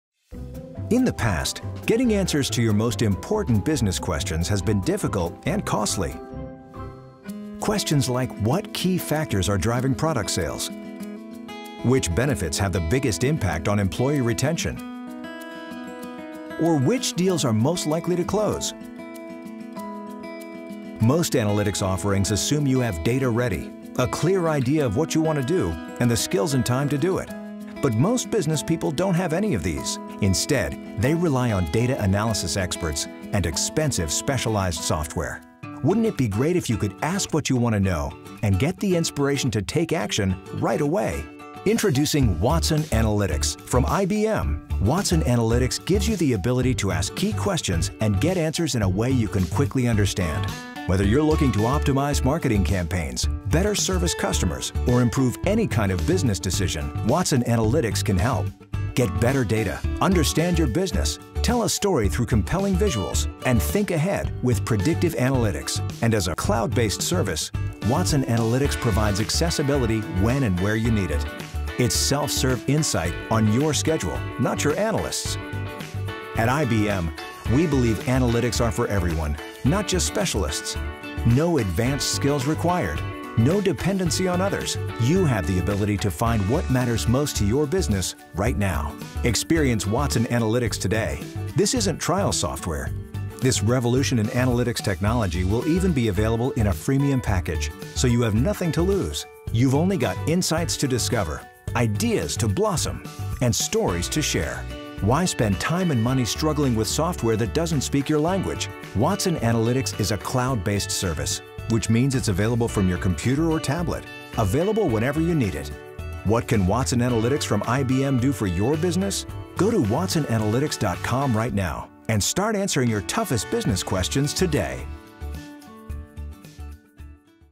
¿Qué es Watson Analytics?
Watson Analytics le da la capacidad de hacer preguntas claves de su negocio y recibir respuestas.
Obtener respuestas de su negocio, Extraer mejores resultados, Comprender mejor su negocio, Anticiparse.